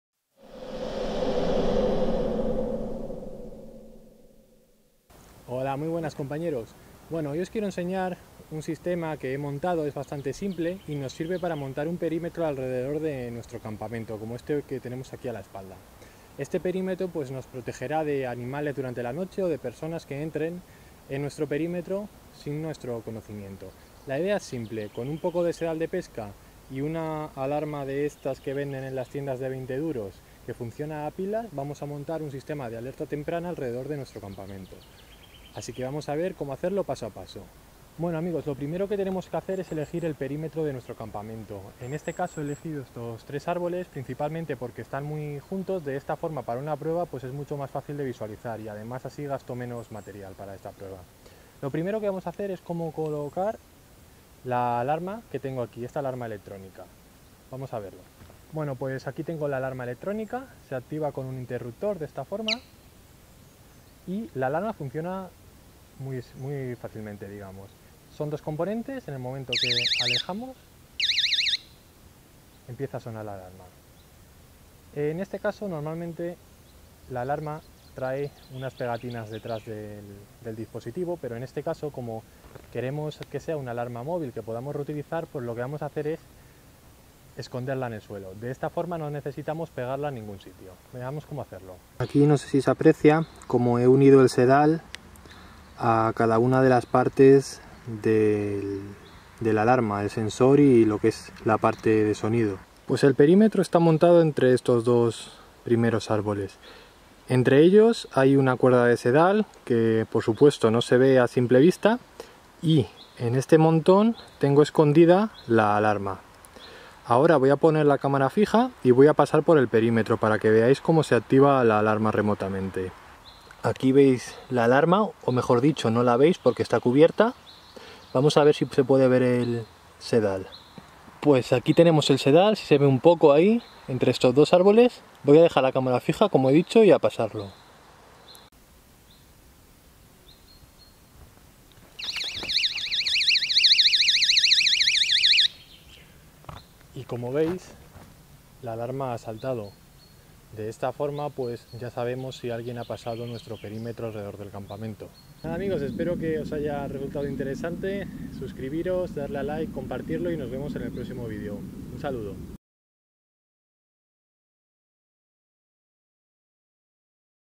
Alarma electrónica para campamento
Cómo instalar tu propia alarma electrónica perimetral para campamento por menos de 3€.

Una sencilla pero funcional alarma que te permite estar alerta de animales o personas que intenten acceder a tu campamento.